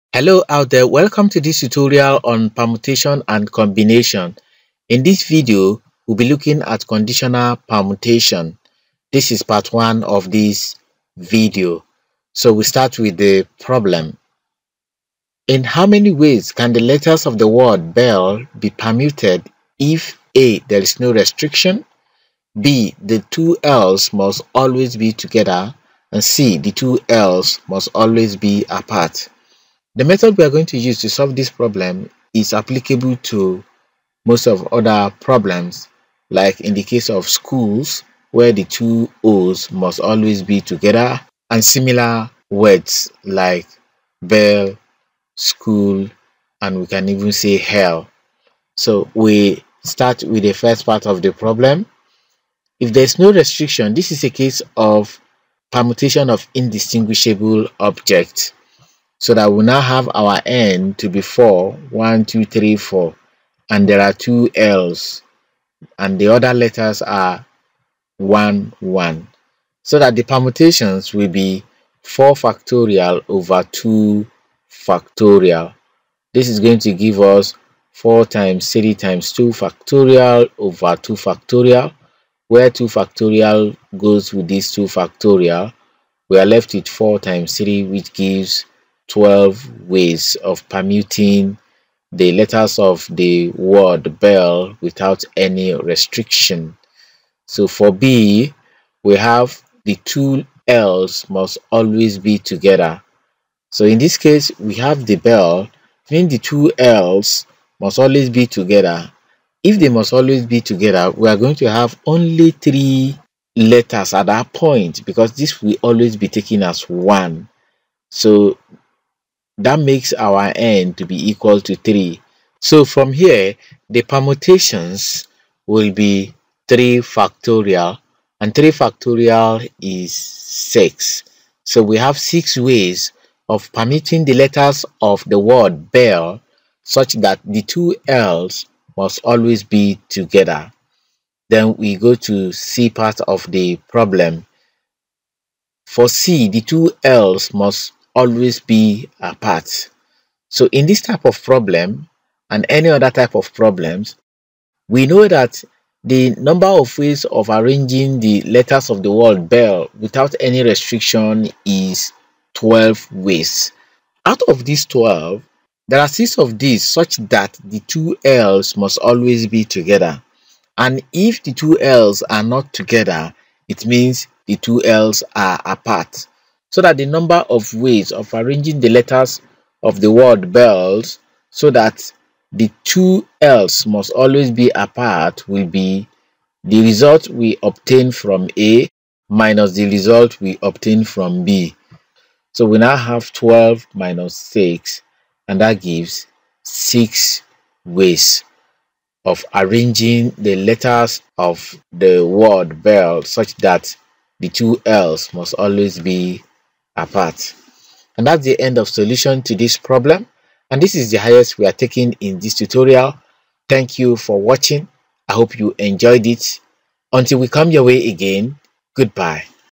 Conditional Permutations 1
This video covers conditional permutation in which letters of the word BELL is discussed so that the two L's are either together or apart.